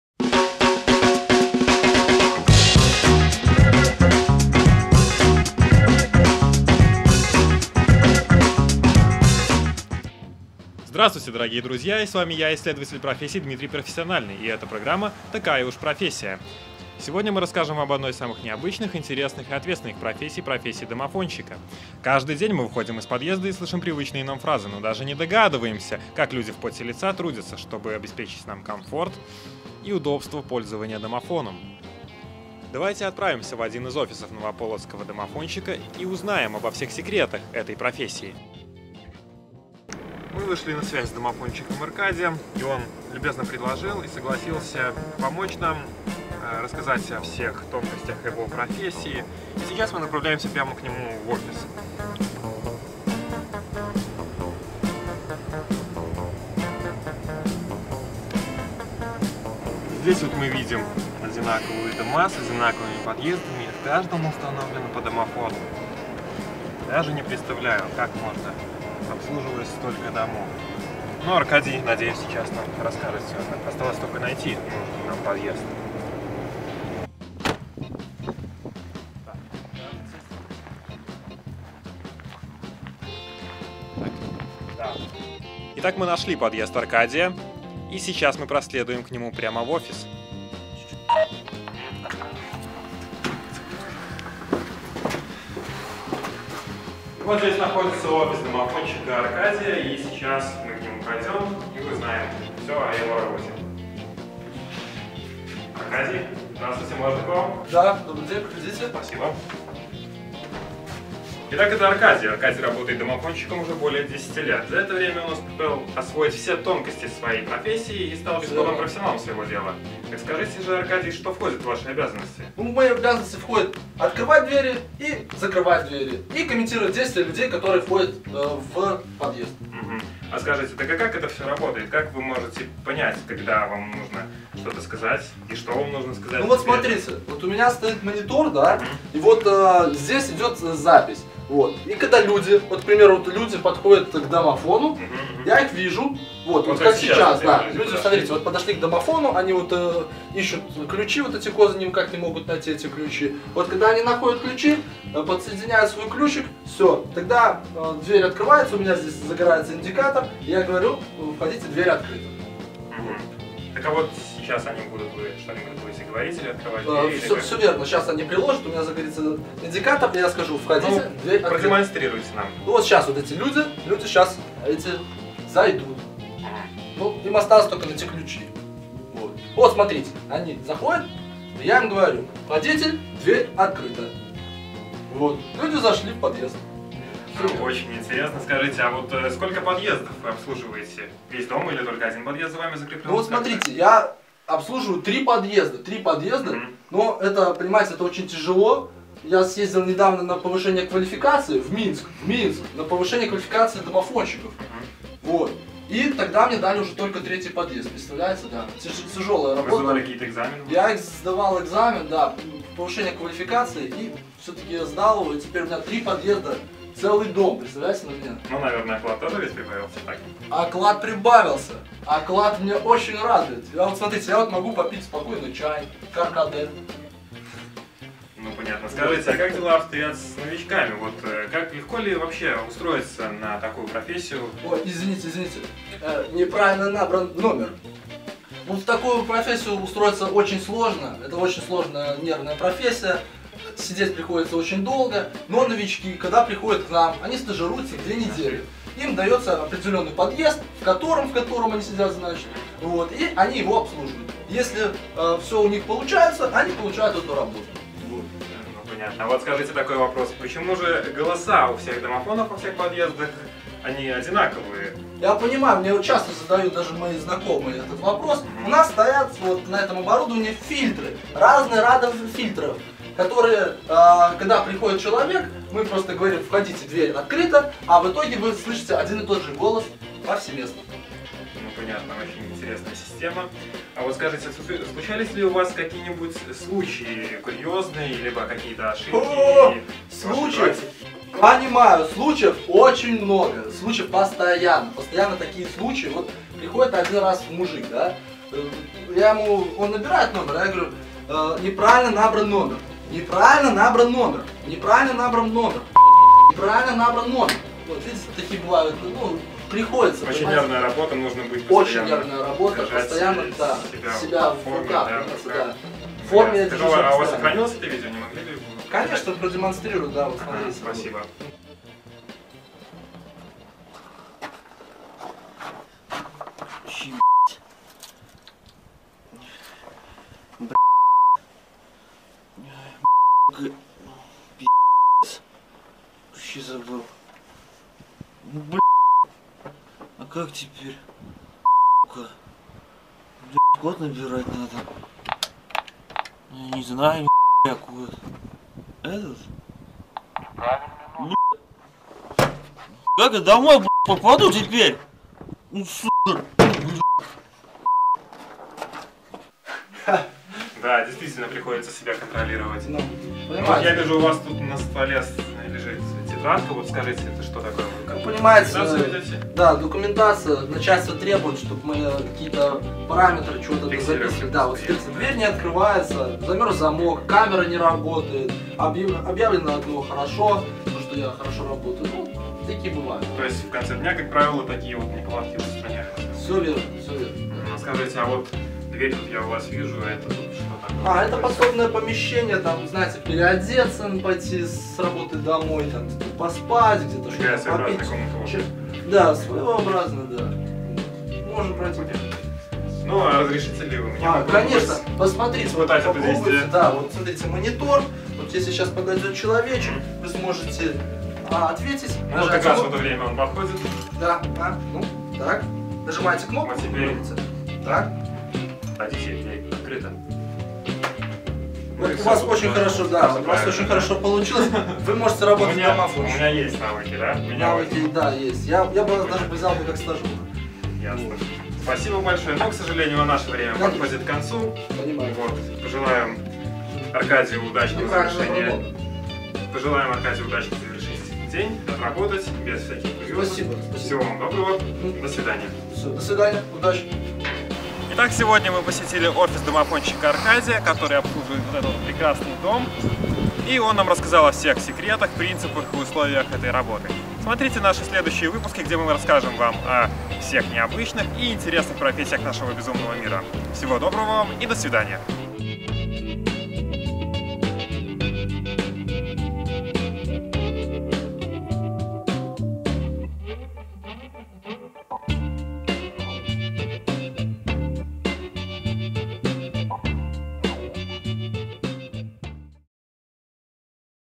Такая уж профессия-Как работает домофон
Проект "Такая уж профессия". Пилотный выпуск. Проект будет представлять собой серию передач о различных "необычных" профессиях современного мира.
Идея:Дмитрий Гурченок
Оператор,монтаж :Сергей Шевцов
Роли:Дмитрий Гурченок, Антон Ковалёв